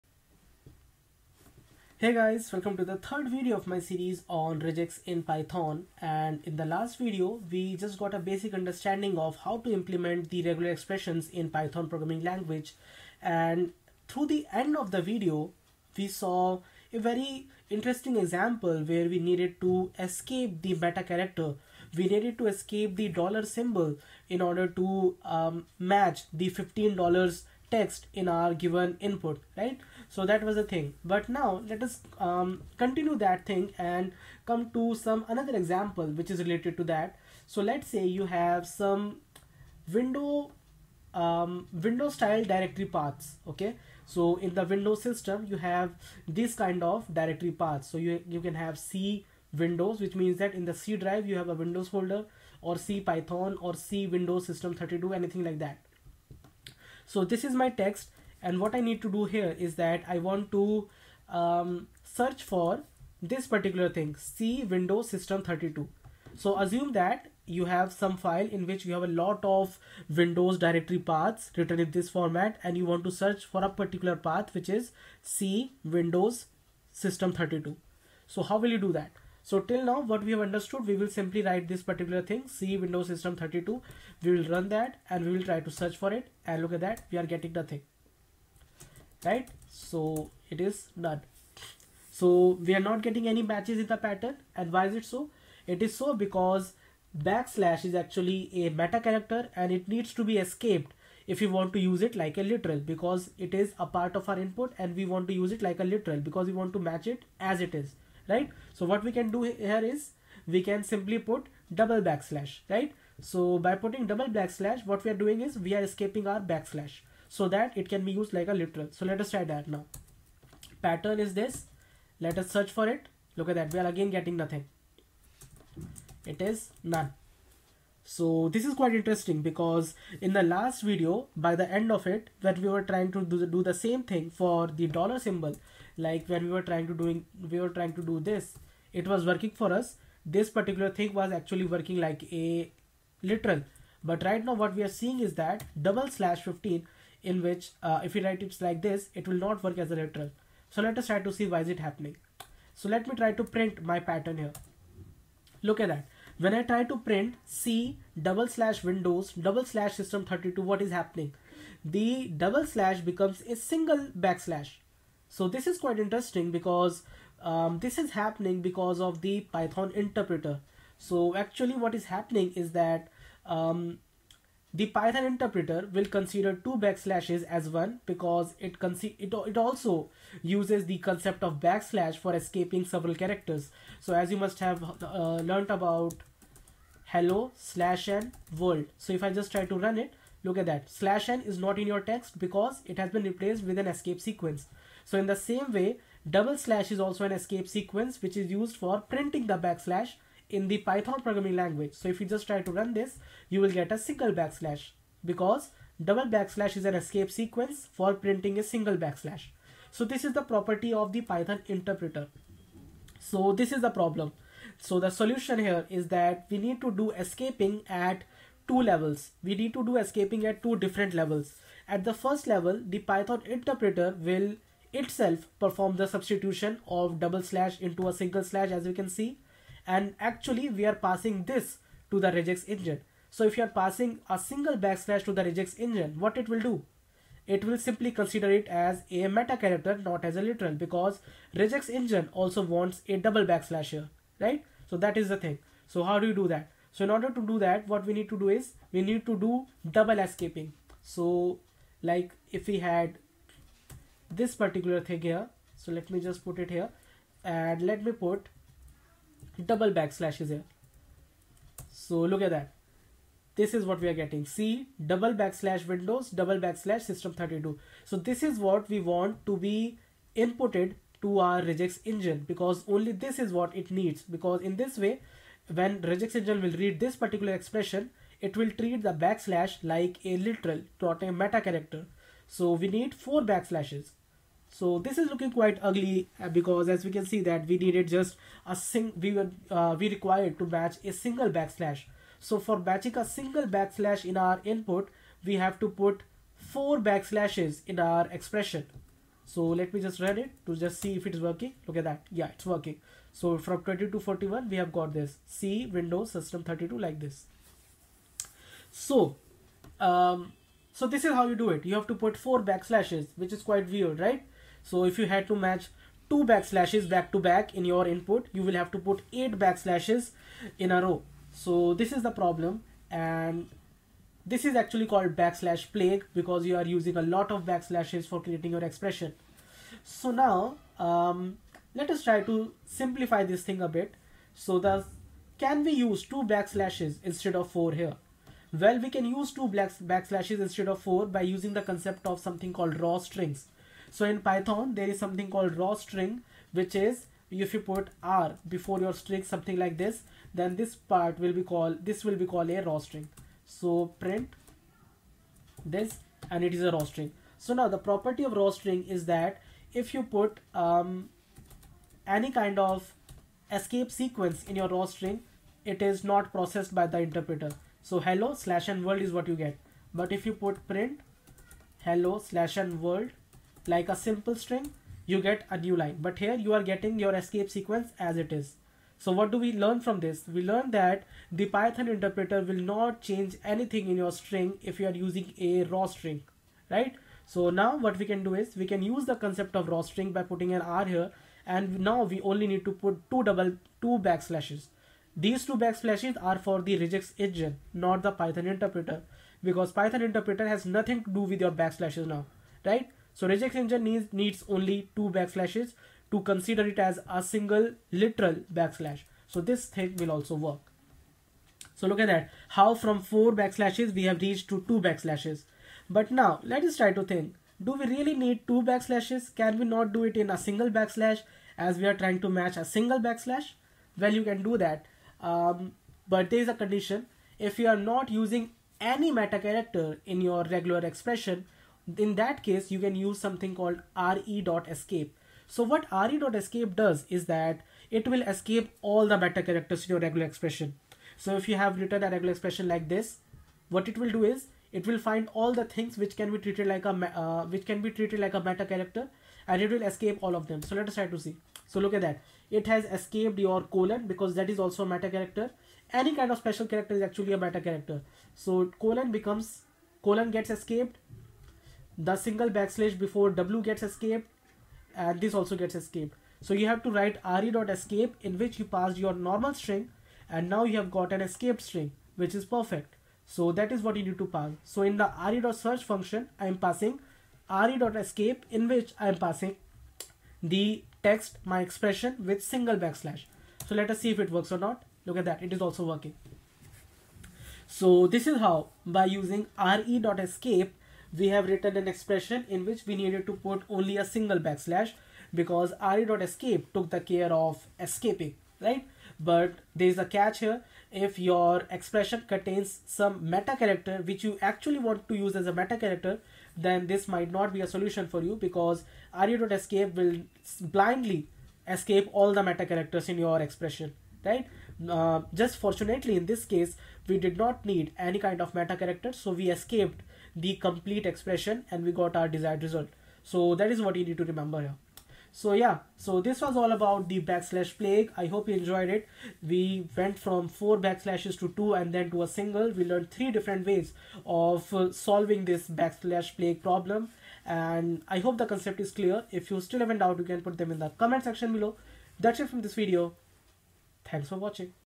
RegEx in Python (Part-3) | Character Classes
This video discusses the basics of character classes in regular expressions.
The character classes (also known as character sets) allow us to define a character that will match if any of the defined characters on the set is present.
In this video, learn about:
1. Character Classes
2. Character Set Range
3. Predefined Character Classes